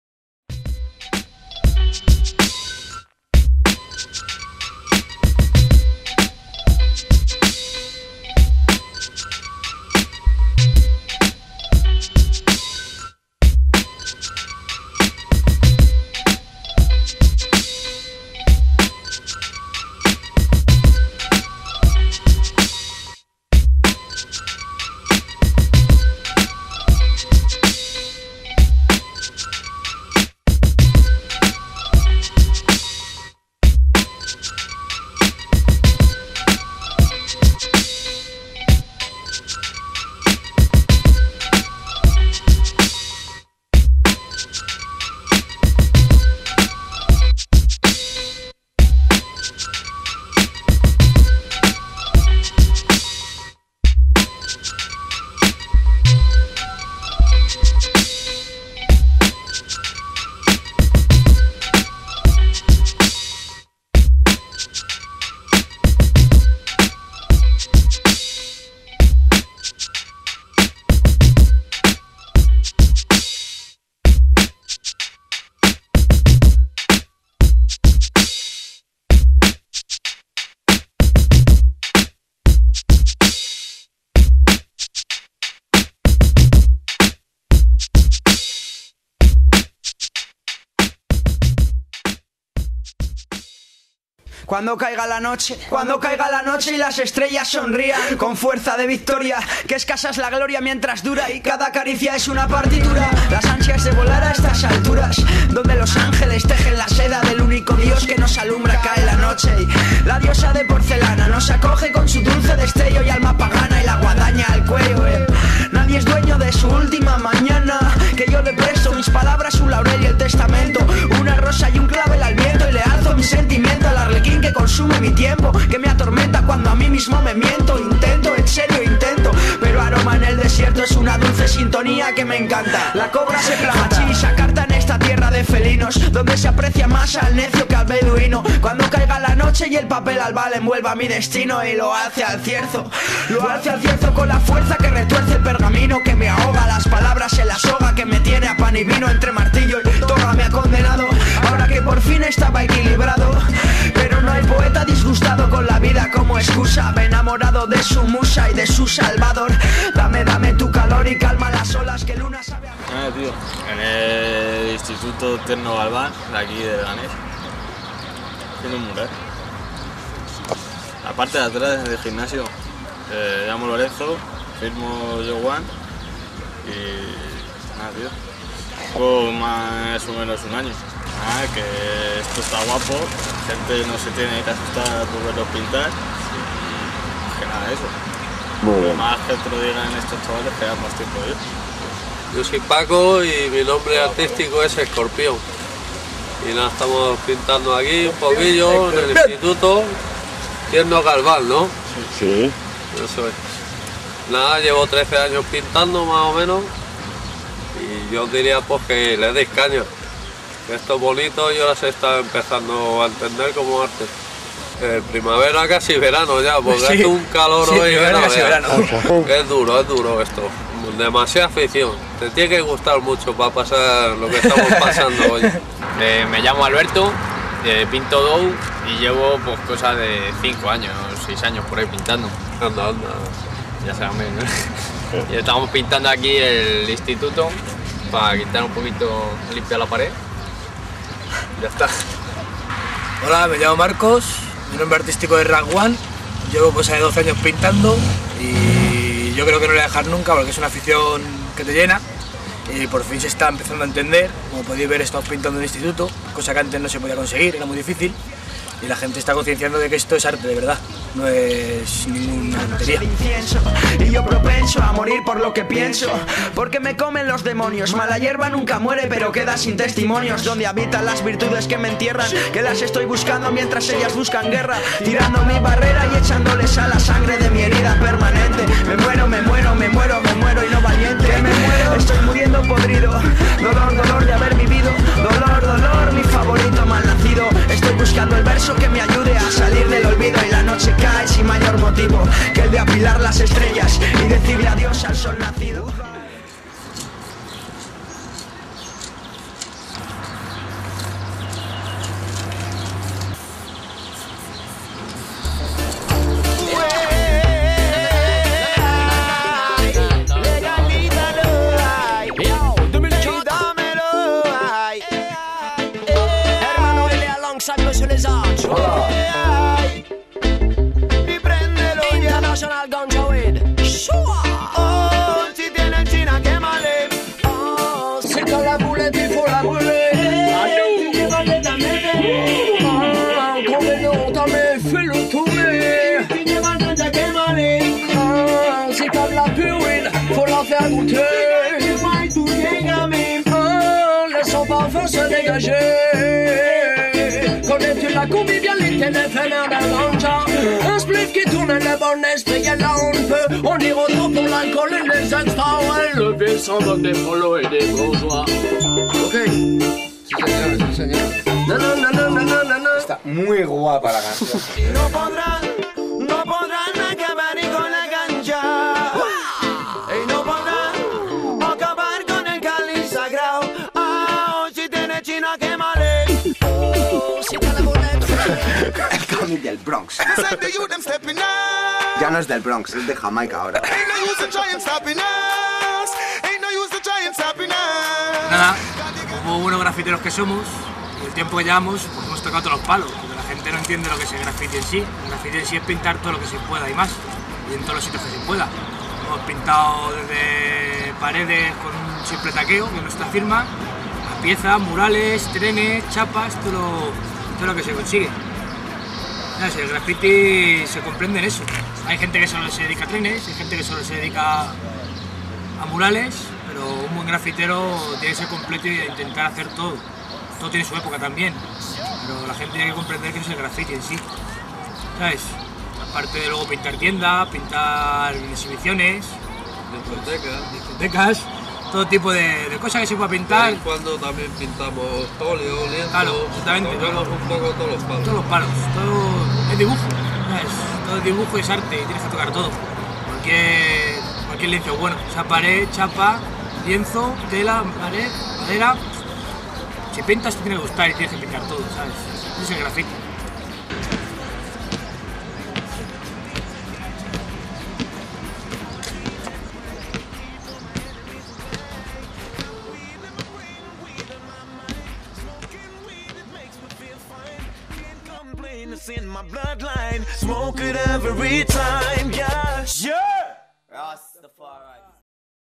Graffity IES Enrique Tierno Galvan 2008
Aqui os dejo un video que tiene ya un tiempecito, de cuando pintamos en 2008 las paredes del instituto. Esta grabado por el profesor de artes Rovira, artistas: DOW, PION, JOY, RAPS. Me trae muy buenos recuerdos, espero que os guste.